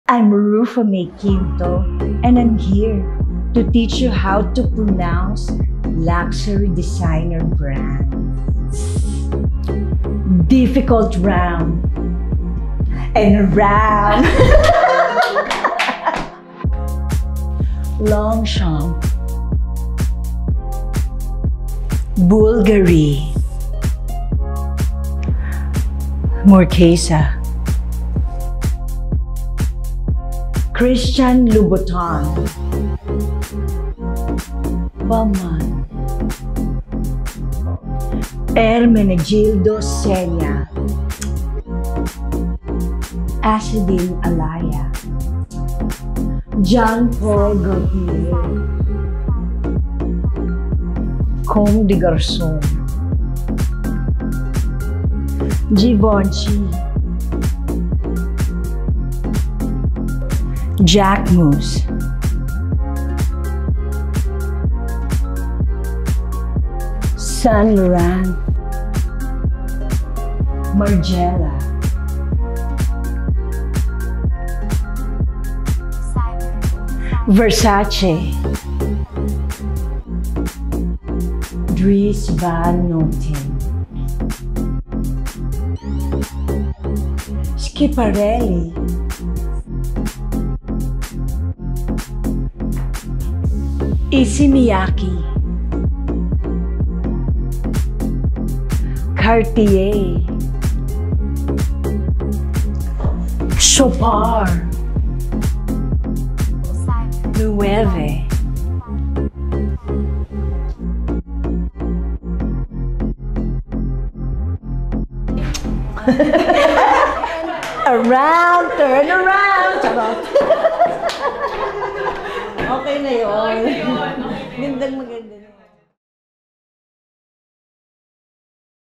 How to Pronounce Designer Fashion Brands with Rufa Mae Quinto | PREVIEW
Back in 2013, Rufa Mae Quinto starred in an iconic Preview video where she schooled us on the most relevant luxury designer brands at the time. A decade later, the comedienne is back and she's ready to level up! This 2023, she's up for a bigger challenge a.k.a. the difficult round, where she'll teach us some of the most mispronounced labels like a true-blue fashion girl—Rufa Mae style.

Press play and learn from the icon herself!

Time Stamps
0:00 Intro
0:16 Longchamp
0:19 Bvlgari
0:23 Marchesa
0:28 Christian Louboutin
0:32 Balmain
0:38 Ermenegildo Zegna
0:42 Azzedine Alaïa
0:46 Jean Paul Gaultier
0:51 COMME des GARÇONS
0:54 Givenchy
0:59 Jacquemus
1:06 Saint Lauren
1:10 Margiela
1:17 Versce
1:21 Dries van Noten
1:27 Schiaparelli
1:33 Issey Miyake
1:37 Cartier
1:42 Chopard
1:46 Leowe